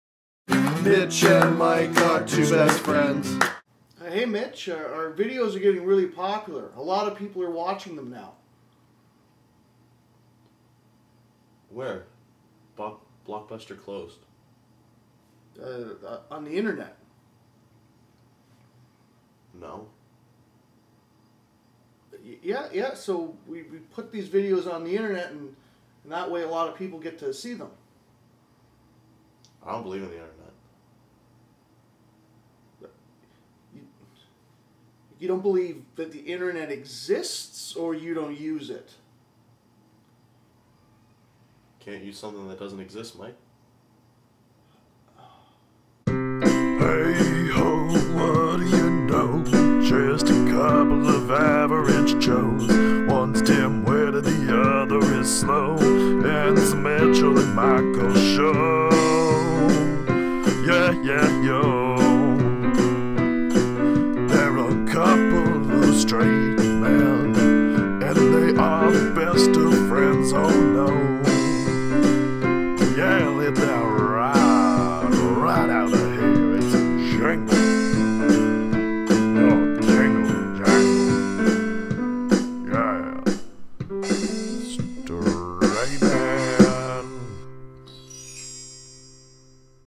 Two Straight Men | Ep. 12 - The Internet
Episode 12 - Two Straight Men starring Mitch & Mike

Mitch and Mike discuss the internet.